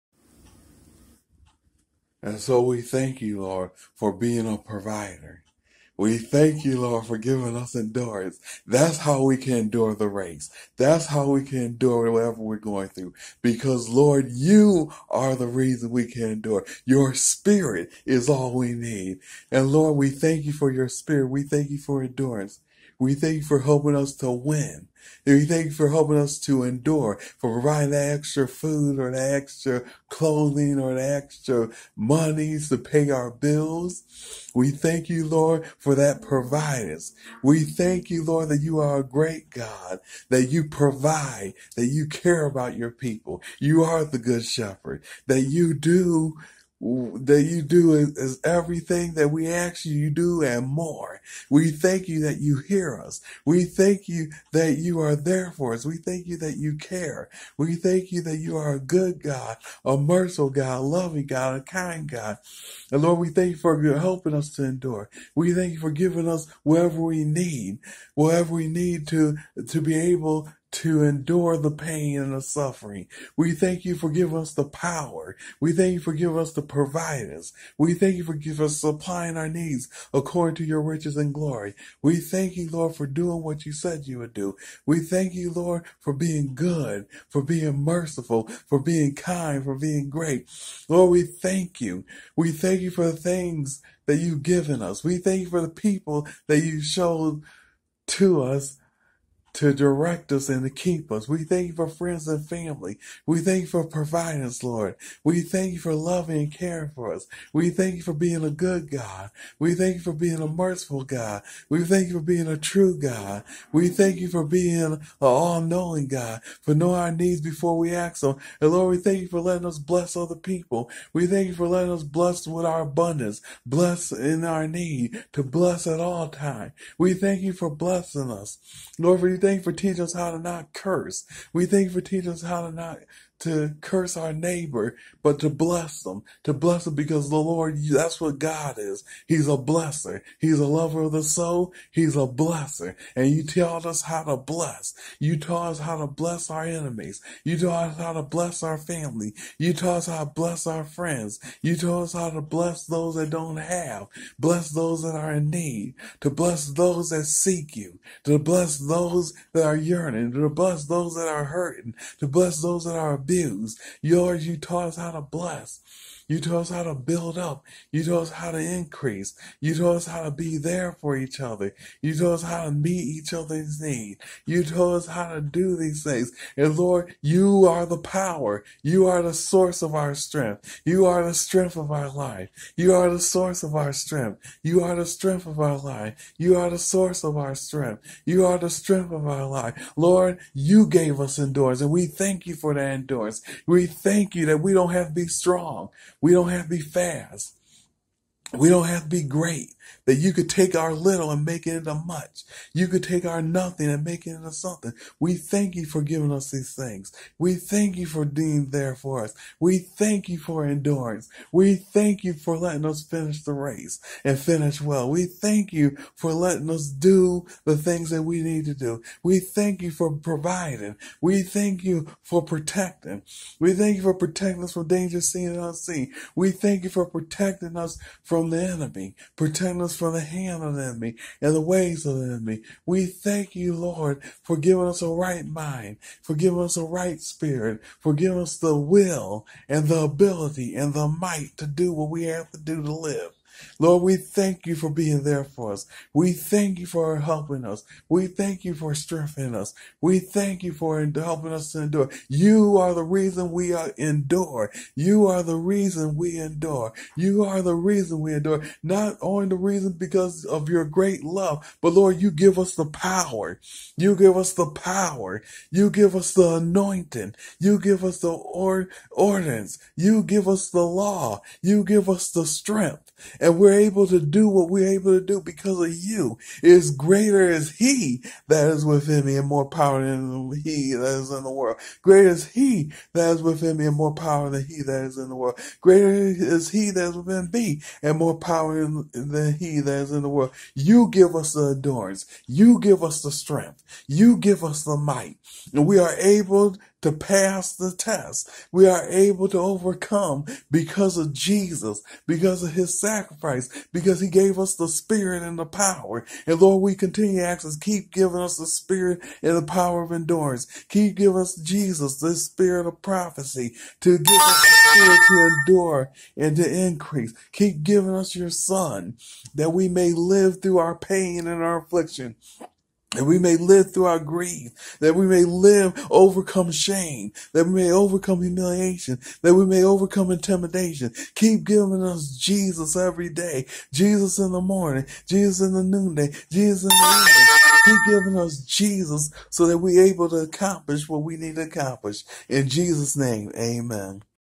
Apostle Matthew Primous prays for Endurance
Apostle and Prophet Matthew Primous is the founder of Interfaith Alliance of Bourgnew, a Church officiated in 2001. He comes from the Israel's Priestly Tribe, the Levites.  His prayers are world famous.